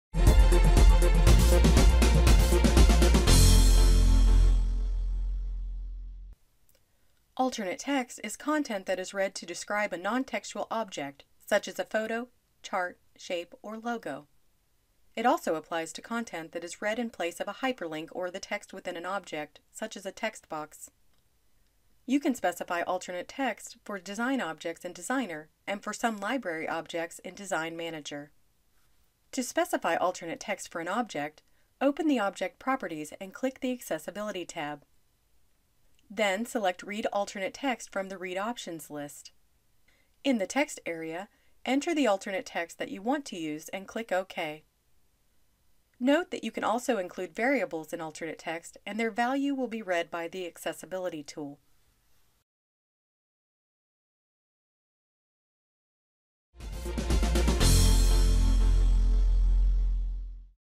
Specifying alternate text | OpenText Exstream 16 Design and Production
Alternate text is text that is read by an accessibility tool when it encounters graphic objects or hyperlinks. Learn how to apply static and variable alternate text to design objects in OpenText Exstream Design and Production.